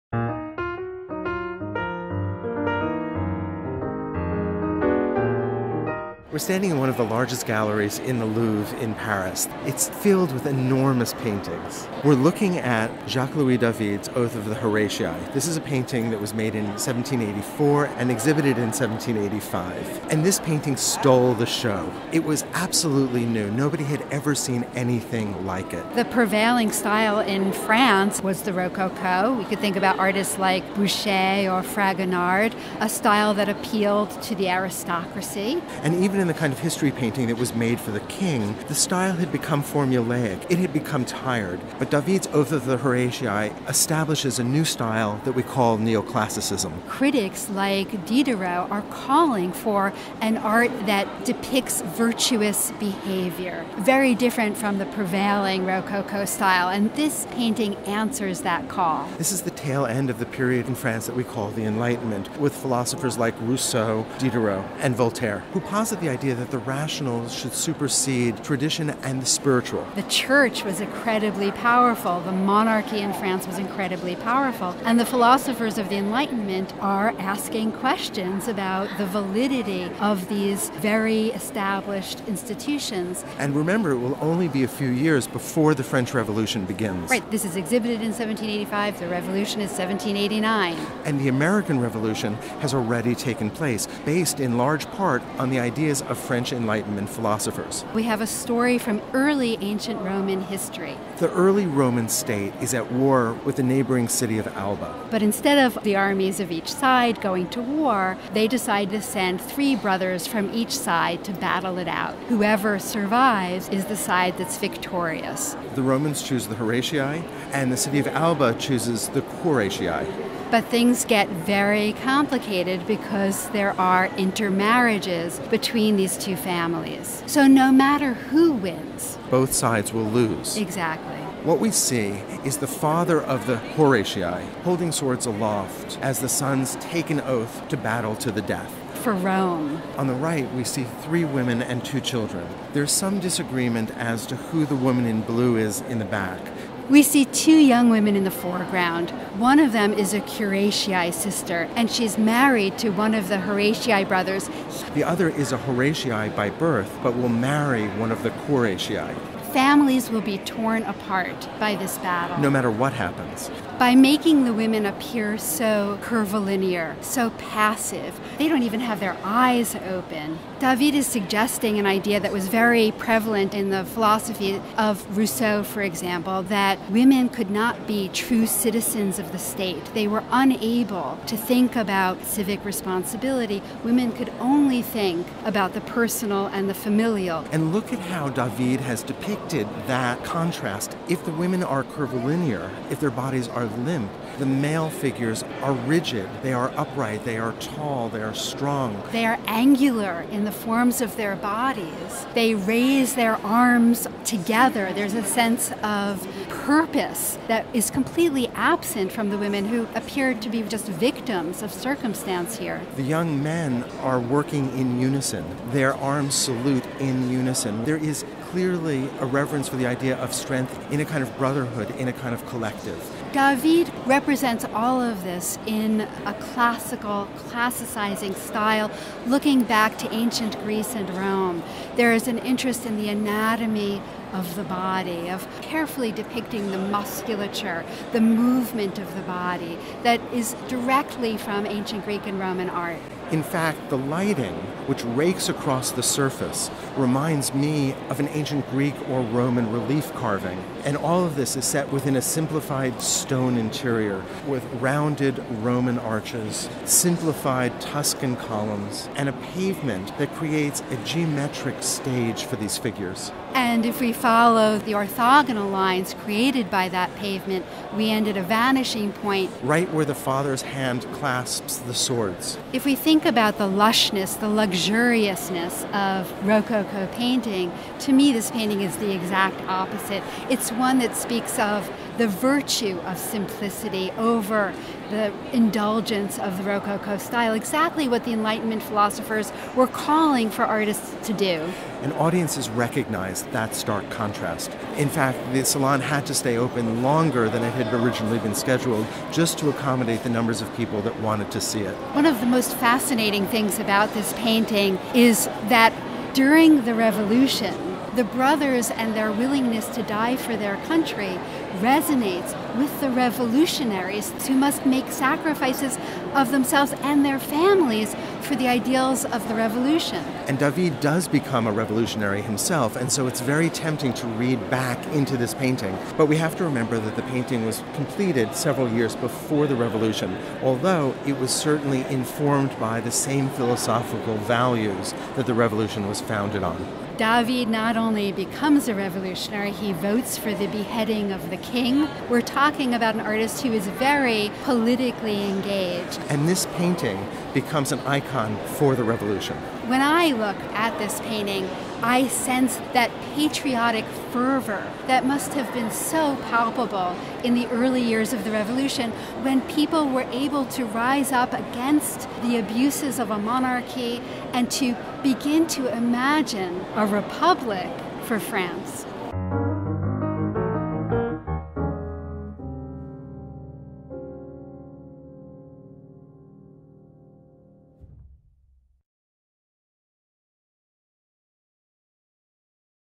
David, Oath of the Horatii
Jacques-Louis David, Oath of the Horatii, 1784, oil on canvas, 3.3 x 4.25 m, painted in Rome, exhibited at the salon of 1785 (Musée du Louvre) Speakers: Dr. Beth Harris and Dr. Steven Zucker